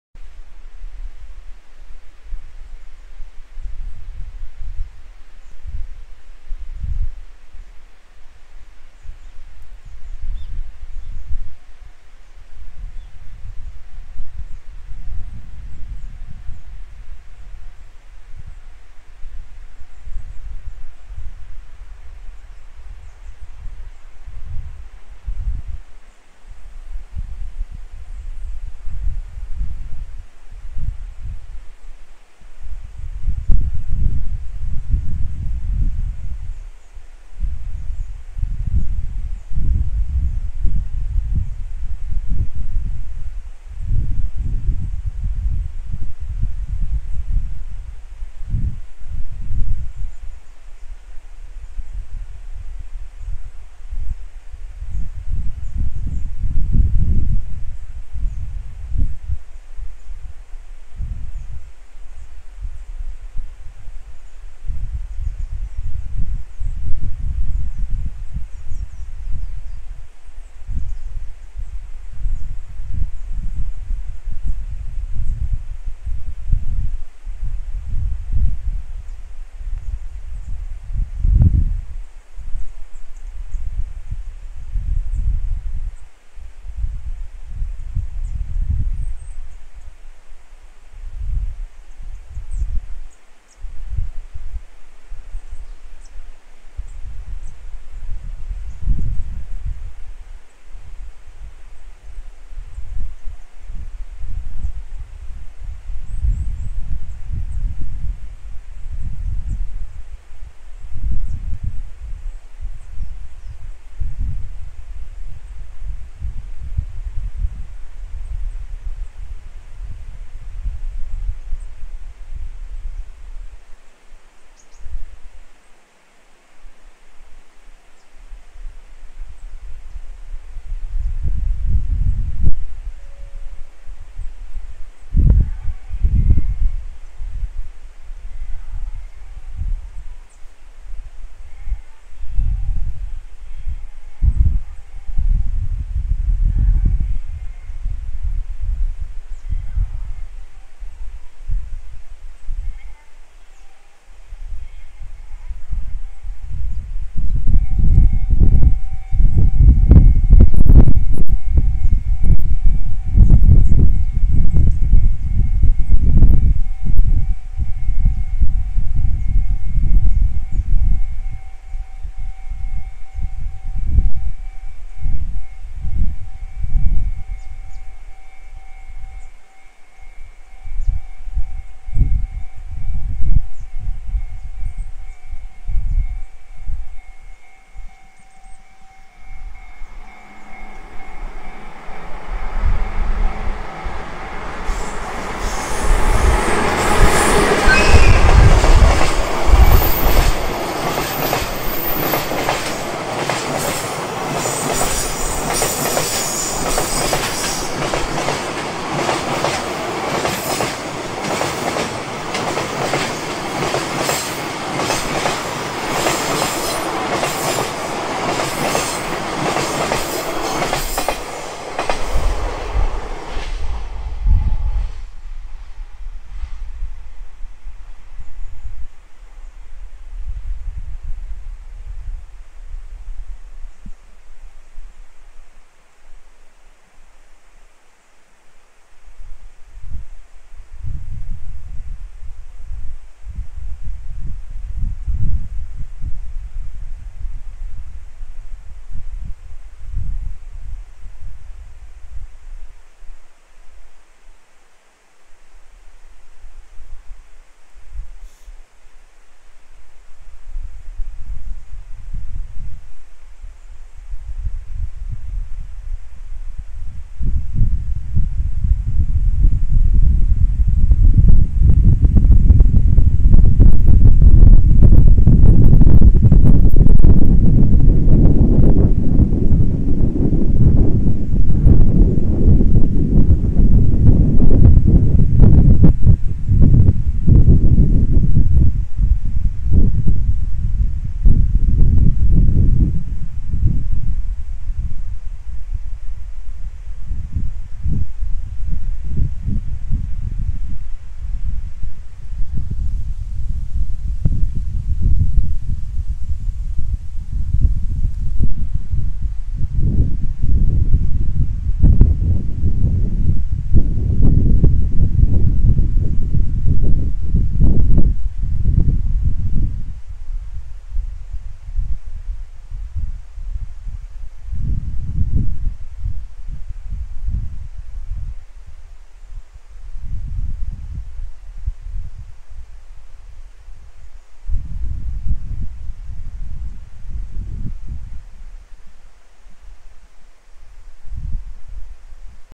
小幌駅上りホームより下り貨物列車
貨物列車の走行音というのはなんと心を穏やかにしてくれるものでしょう。小幌駅で聞くと、狭い空間に響き合いそれはまた格別なものでした。